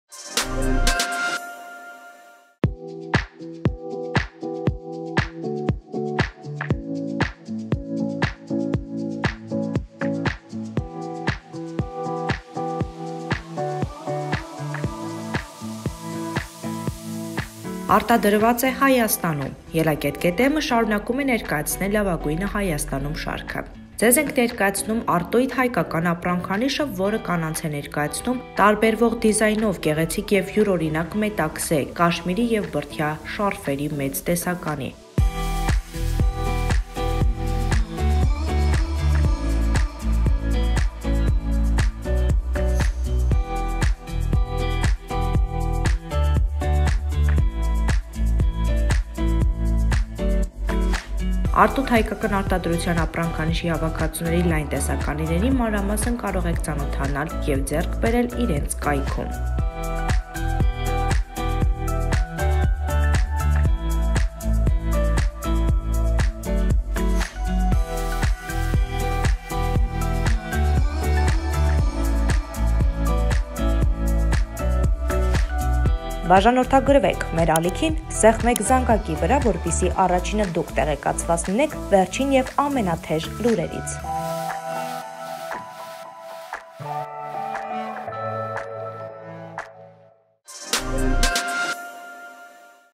Artuyt հայկական ապրանքանիշ
Artuyt հայկական ապրանքանիշը հայ կանանց է ներկայացնում լավագույն որակով և դիզայնով, գեղեցիկ և յուրօրինակ, մետաքսե, կաշիմիրի, բրդյա շարֆեր։

 Մանրամասն՝ տեսանյութում